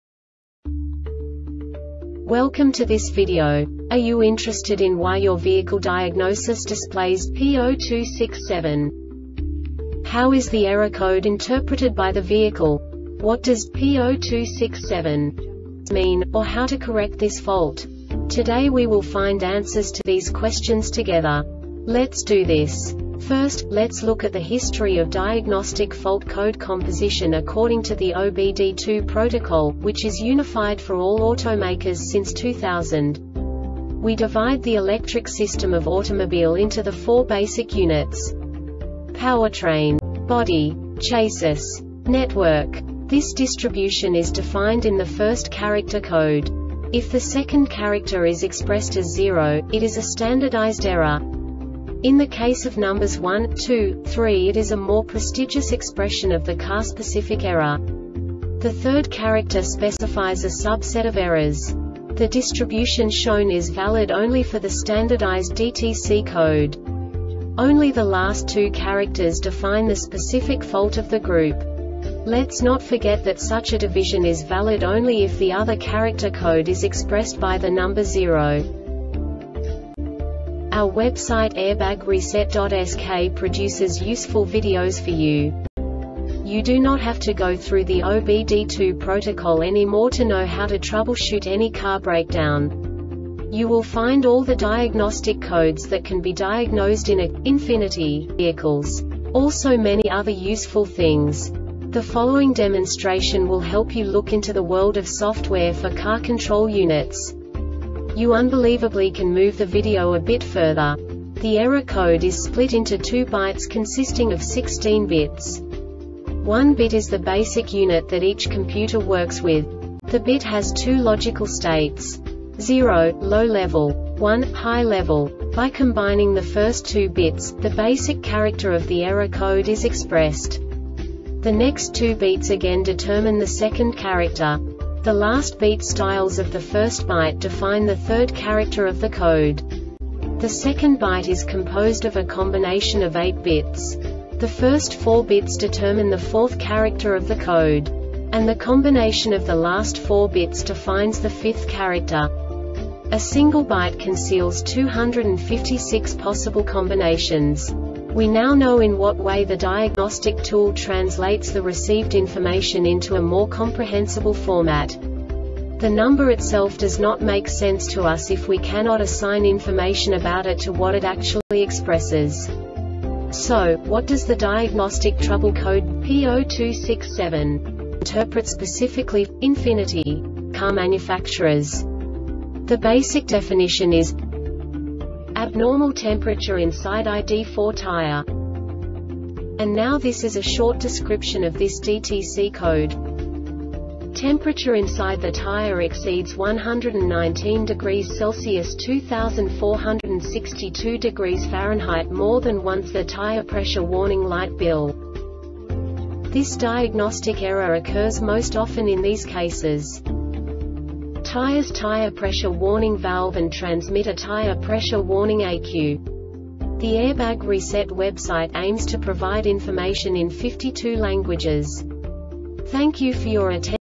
DTC Ford P0267 Short Explanation
Contents:
0:21 Basic DTC analysis according to OBD2 protocol standard.
1:48 Insight into programming
2:58 A diagnostically understandable description of the Diagnostic Trouble Code
3:05 Basic error definition

Make:
Ford
Code:
P0267
Definition:
Abnormal Temperature Inside ID4 Tire
Description:
Temperature inside the tire exceeds 119°C (246.2°F) more than once.  The tire pressure warning light bilnks.
Cause:
 Tires Tire pressure warning valve and transmitter Tire pressure warning ECU